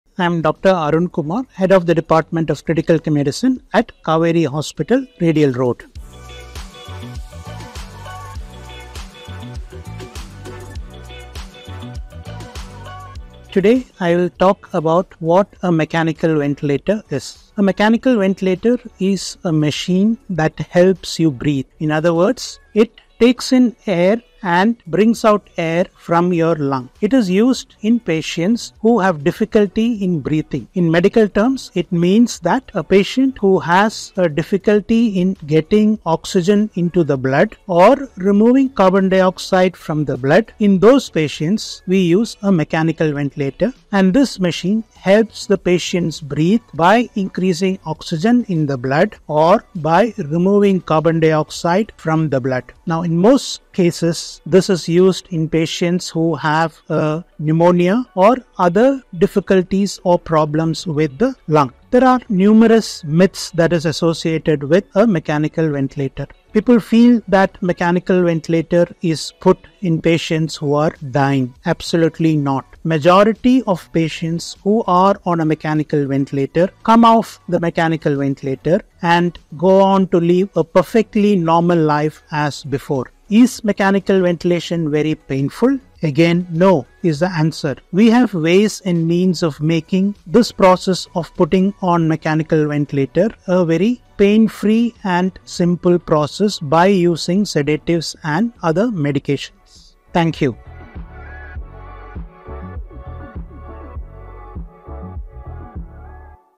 Mechanical Ventilator and Myths Surrounding It
Mechanical Ventilation: Dispelling the Myths 💨

Mechanical ventilators are lifelines for many critically ill patients. Let's debunk some common misconceptions about these devices.

Key points:

Not a death sentence: Ventilators are used to support breathing, not hasten death.
Temporary support: Ventilators are intended to be temporary, aiding recovery.
Not painful: Patients are typically sedated during ventilation.
Not for everyone: Ventilators are used based on medical necessity.
Trust in healthcare professionals: They'll make informed decisions about your care.